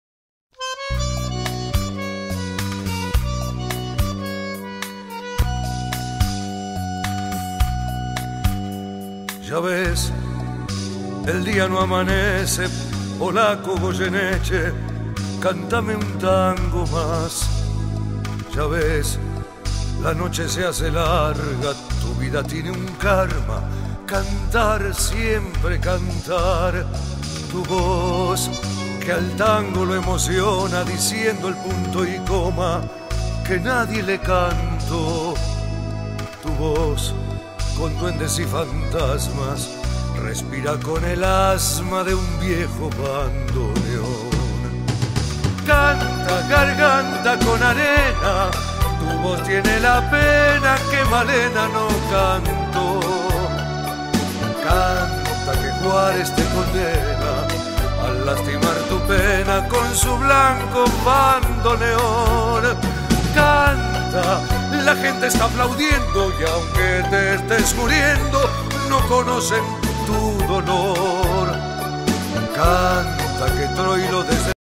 Tango eric V1
Vidéo réalisée par Acopart
d'après l'oeuvre picturale huile sur toile " TANGO " du peintre Eric Tepaz.